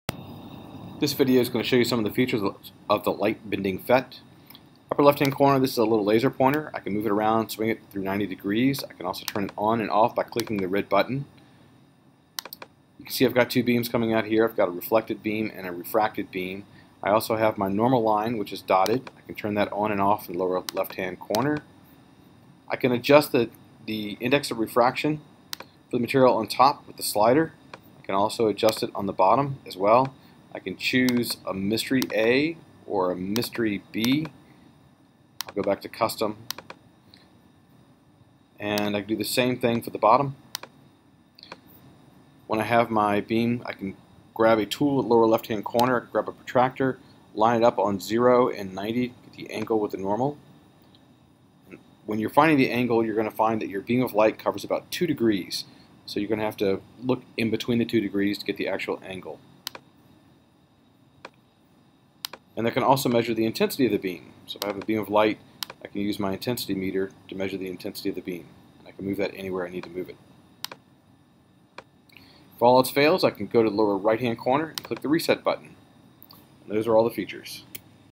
Bending Light PhET Tools explained
This vied describe the basics tools for a light bending activity.